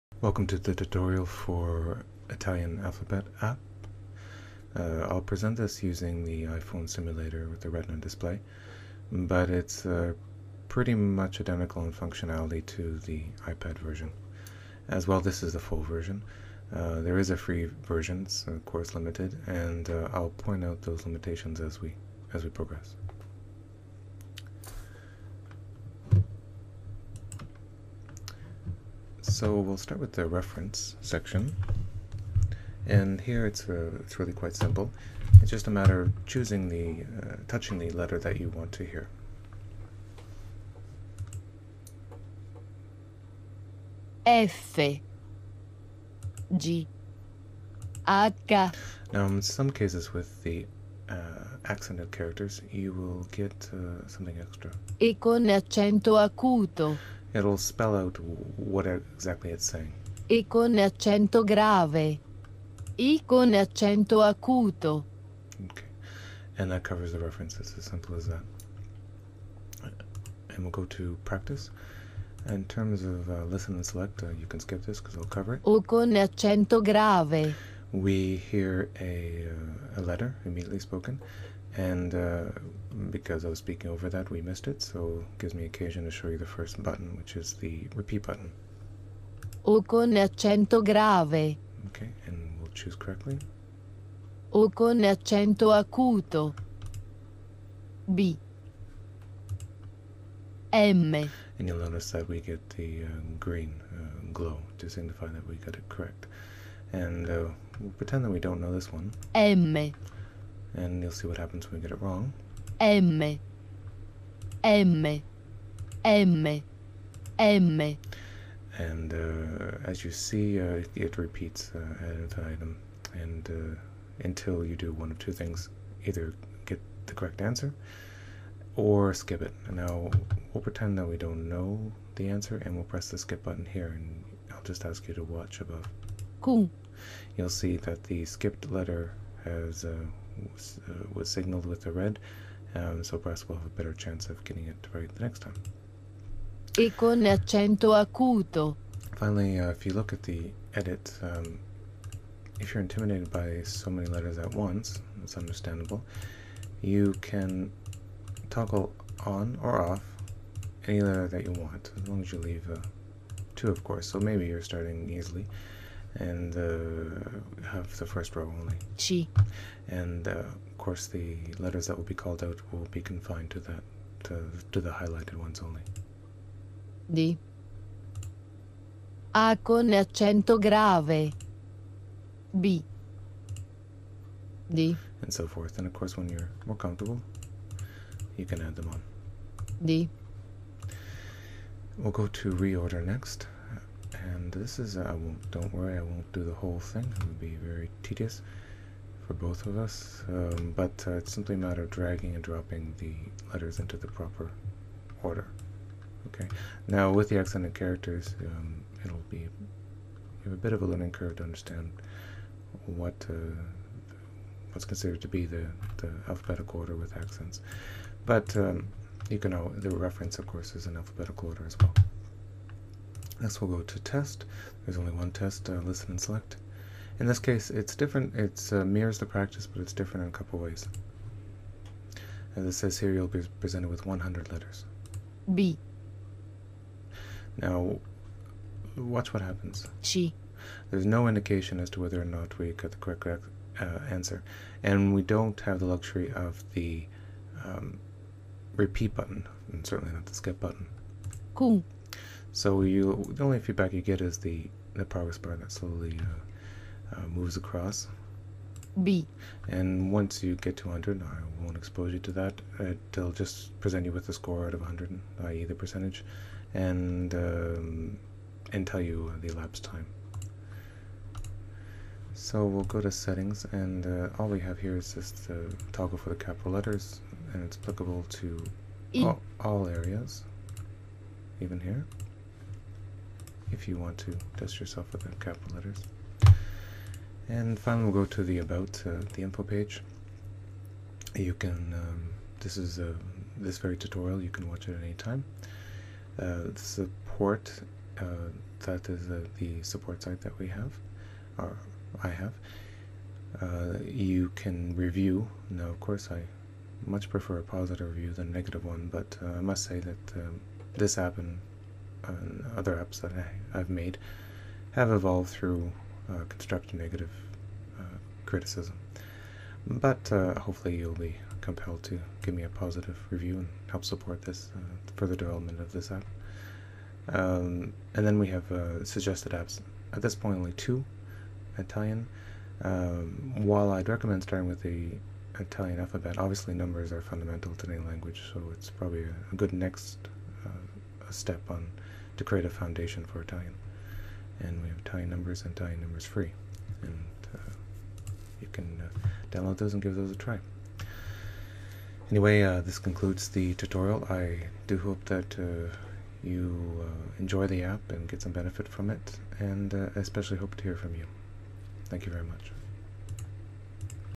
Italian Alphabet App
Tutorial for version 1.50 of the Italian Alphabet app by Erasmos, now available for the iPod Touch, iPhone and iPad. Covers both the Paid and Free versions. You can download the full version or the free, demo version If you interested in volume sales